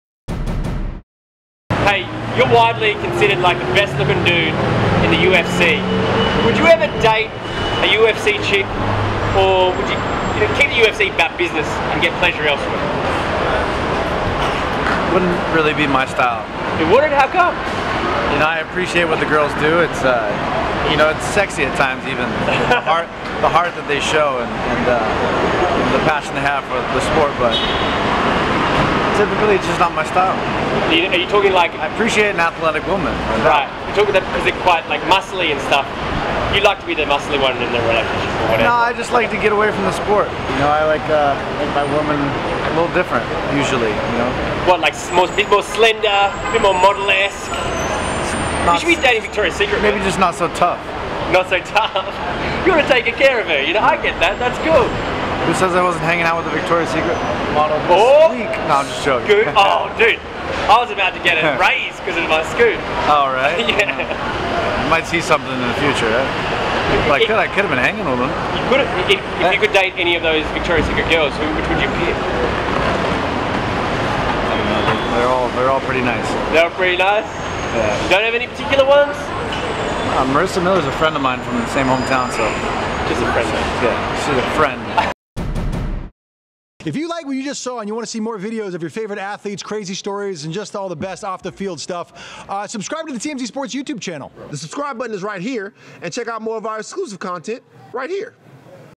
Luke Rockhold: I Don't Date Chick Fighters, 'Too Tough for Me' | TMZ Sports
Don't worry about Luke Rockhold dippin' his pen in the company ink. Despite nailing all sorts of hot women all over the world -- famous ones, wannabe famous ones, you name it -- the UFC star says he draws a hard line when it comes to banging female fighters.